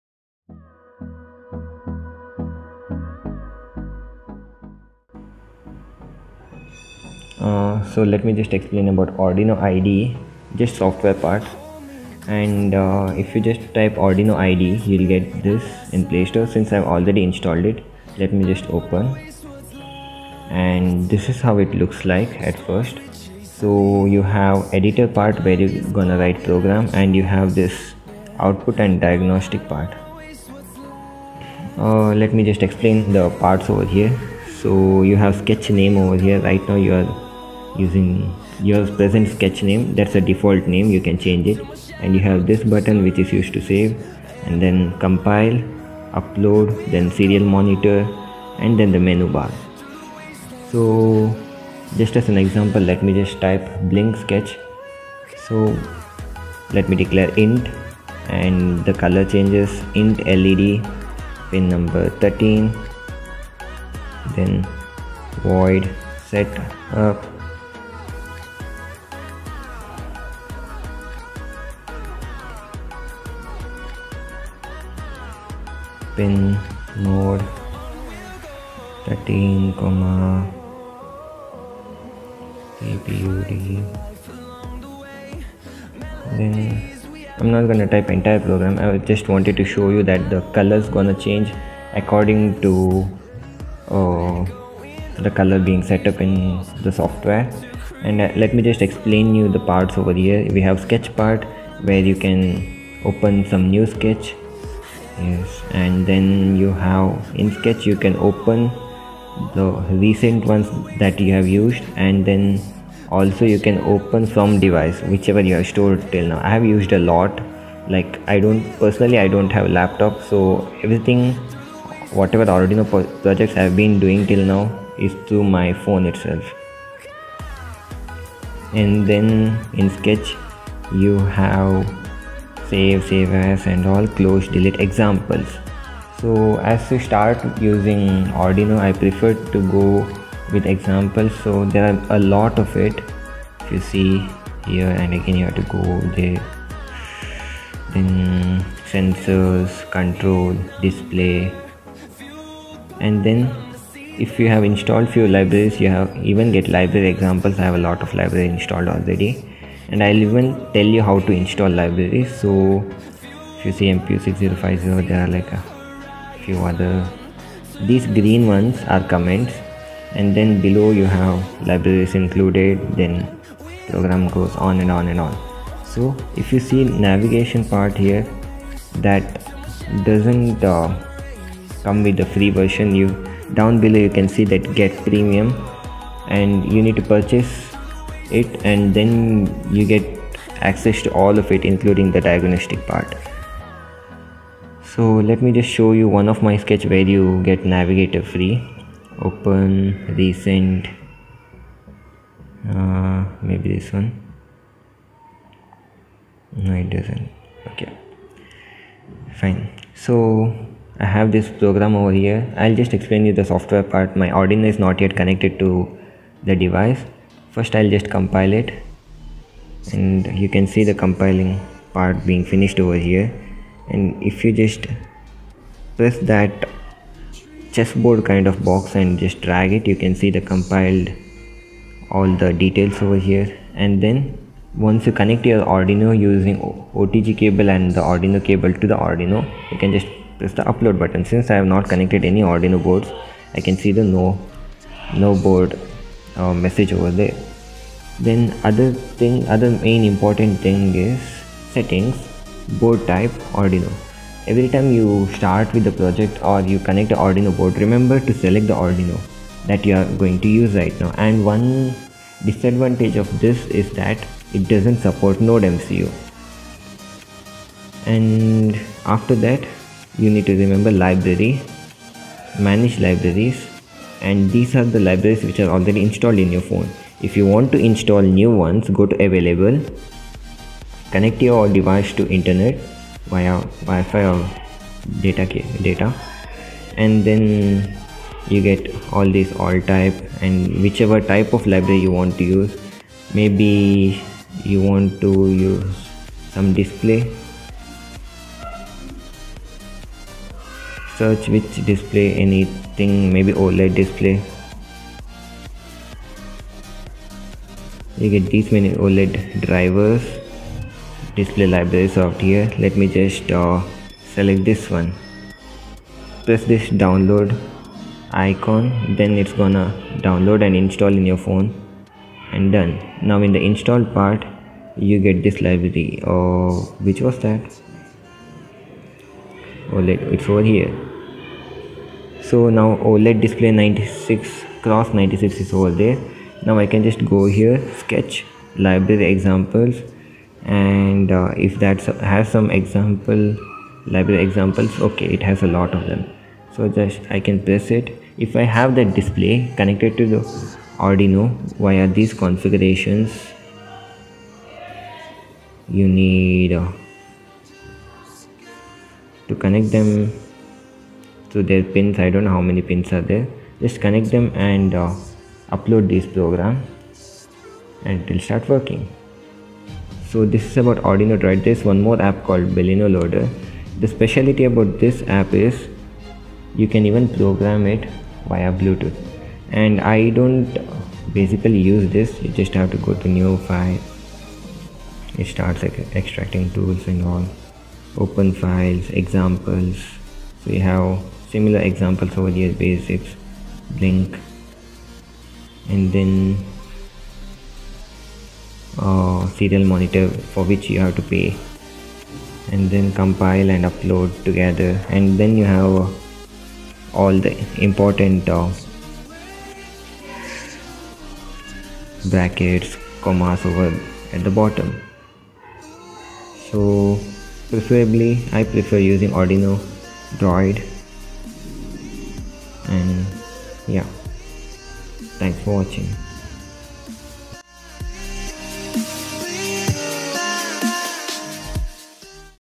How to use ArduinoDroid and Bluino loader | ARDUINO
Hello Friends,
This is a short video explaining mobile app called ArduinoDroid and Bluino loader.

You need not have laptop to do Arduino projects.. these mobile applications will help you to code Arduino